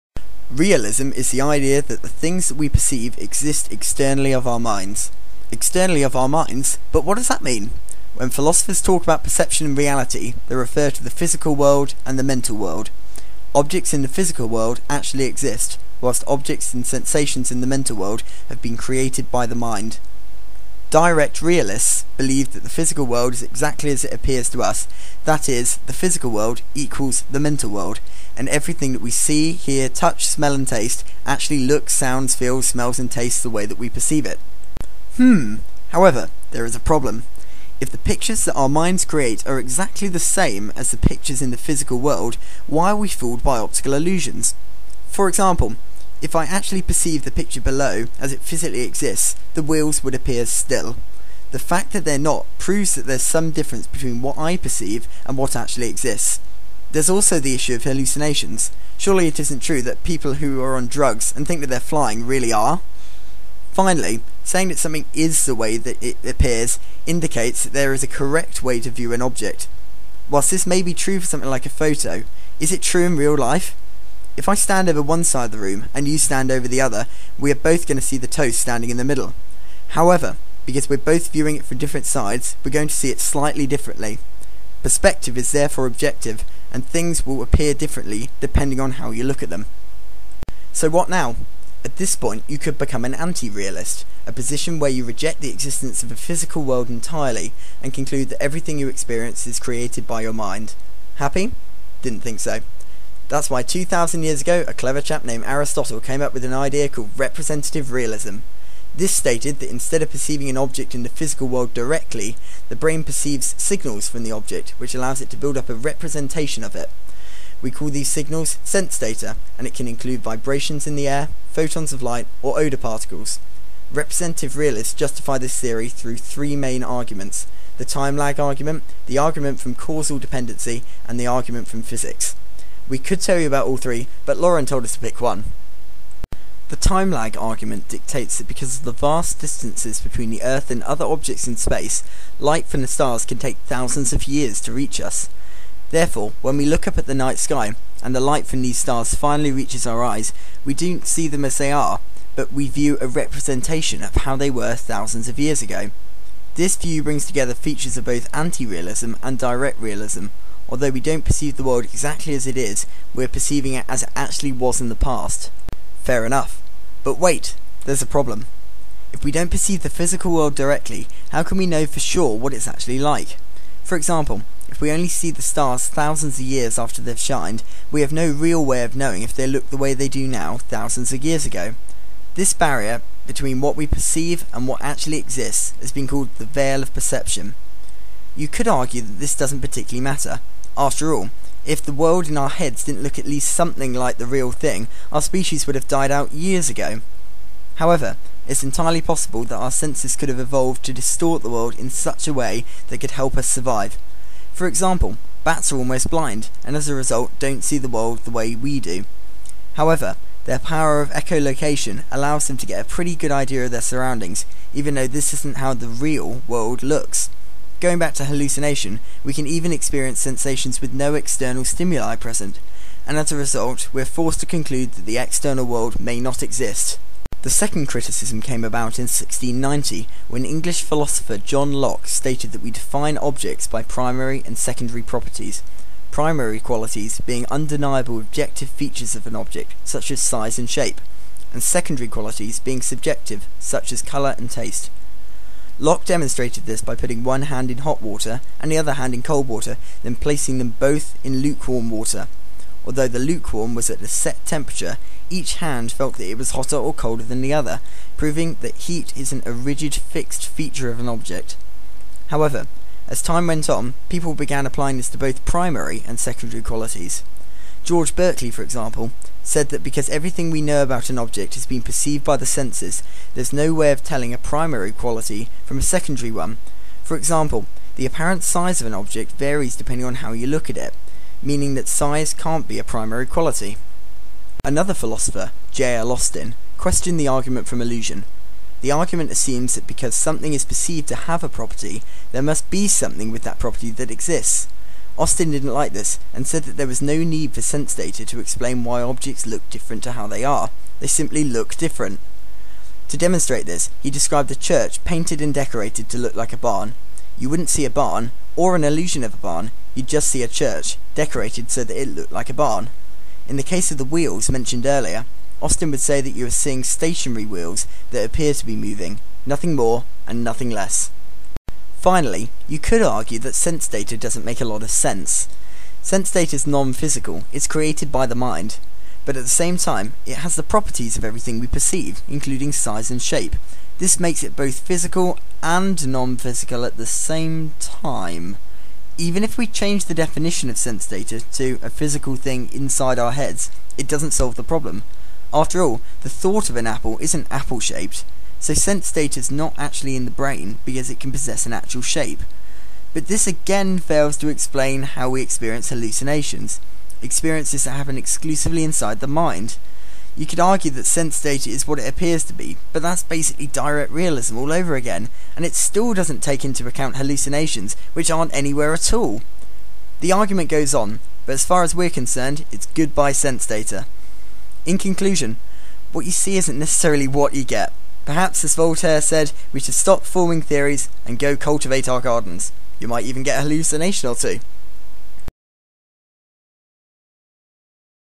Representative Realism in Plain English
A film created to demonstrate representative realism, as revision for AQA AS Philosophy.

Created by David Comley and Katherine Haralampous